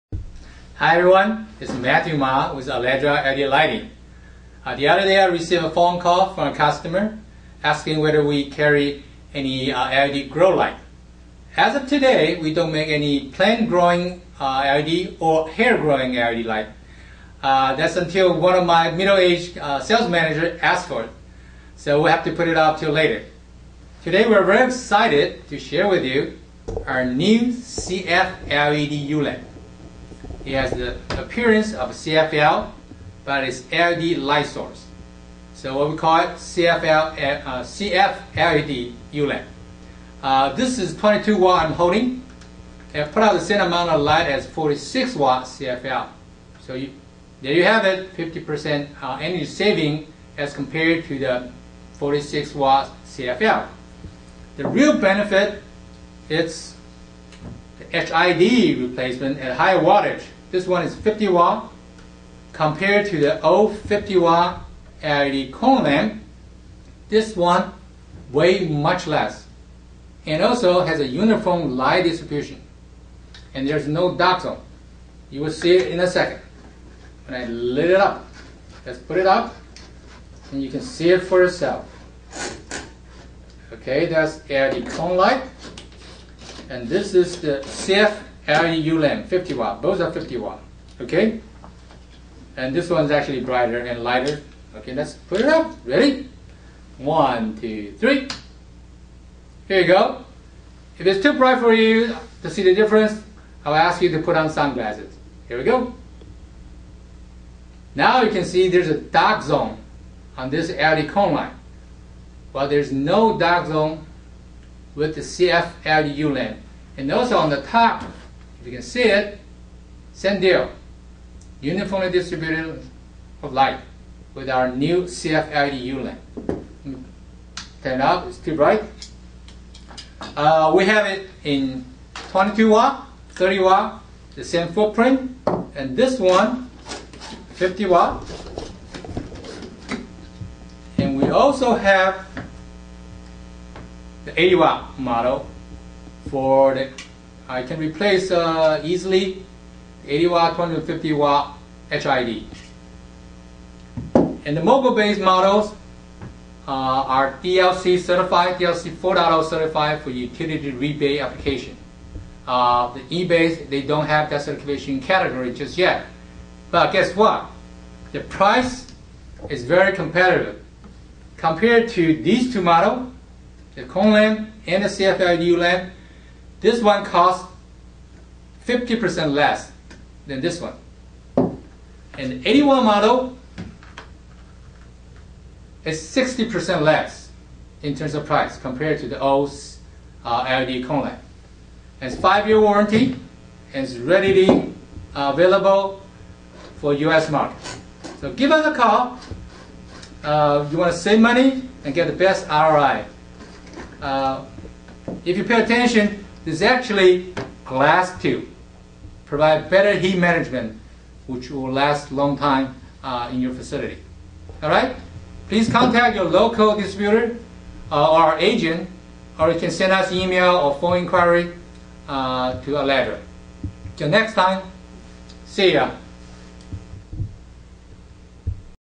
The Most Cost-effective HID Retrofit Option
Aleddra's patent-pending CF-LED U-lamp is the most cost-effective retrofit option for HID lamp. It costs 50% less than the LED corn light, with lesser weight and longer life. The E39 (mogul base) models are DLC 4.0 qualified for utility rebate.